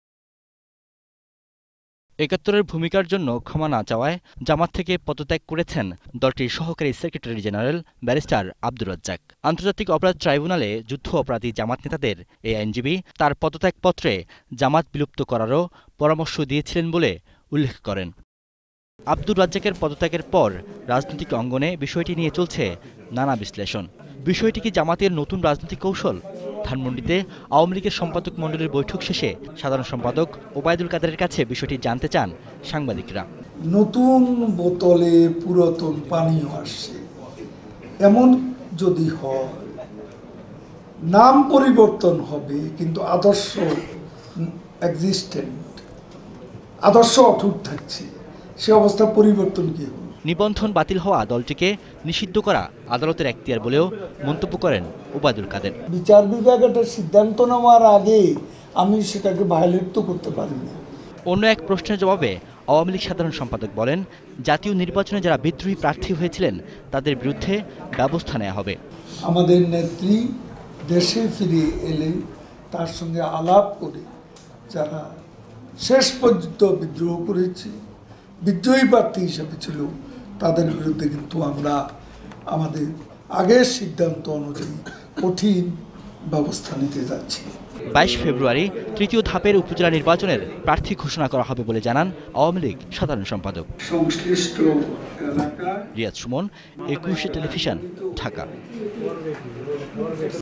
জামায়াত তার নীতি আদর্শ কখনোই বদলাবে না | ETV News
প্যাকেজের বিষয়: জামায়াত তার নীতি আদর্শ কখনোই বদলাবে না
প্রতিবেদক: রিয়াজ সুমন।

নাম পরিবর্তন করে রাজনীতিতে আসলেও যুদ্ধাপরাধী দল জামায়াত তার নীতি আদর্শ কখনোই বদলাবে না বলে মন্তব্য করেছেন আওয়ামী লীগ সাধারণ সম্পাদক ওবায়দুল কাদের। দলটির নিবন্ধন বাতিল হয়েছে, এখন রাজনৈতিক দল হিসেবে নিষিদ্ধ করা আদালতের এখতিয়ার বলেও জানান তিনি। এদিকে, জাতীয় নির্বাচনে যারা বিদ্রোহী প্রার্থী হয়েছিলেন তাদের বিরুদ্ধে দলীয় সিদ্ধান্ত নেয়া হয়েছে বলেও জানান ওবায়দুল কাদের।